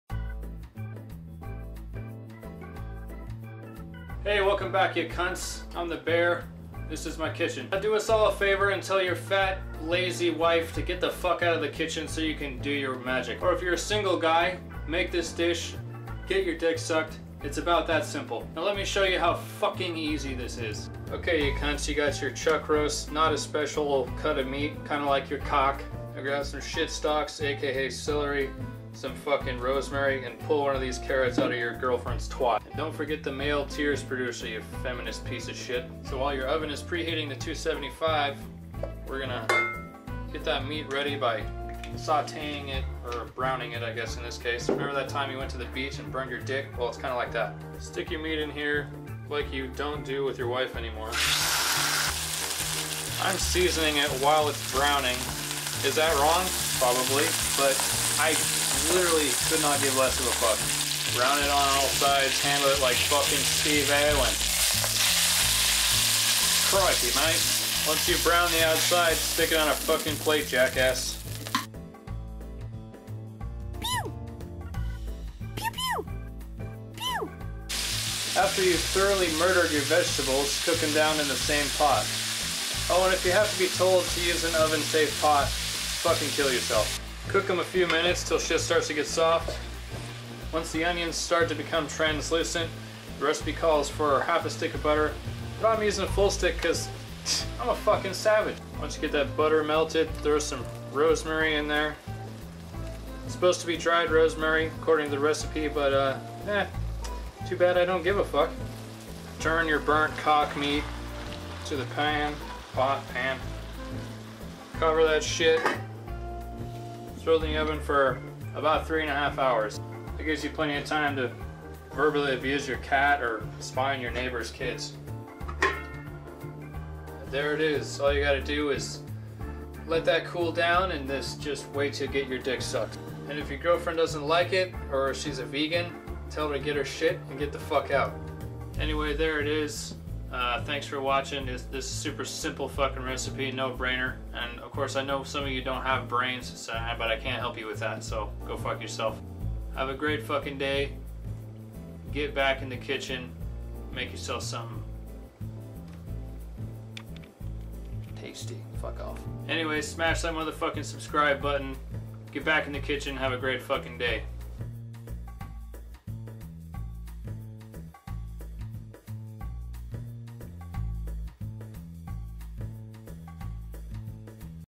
How To: Freaking Easy Pot Roast
Warning: Adult Language
This week I bring you a pot roast that is nothing special to cook but is so fucking delicious that anyone within a block radius will want to go gay for you just to eat it. If not, go fuck yourself, or not...whatever.

Any brand names shown in this video are purely by coincidence.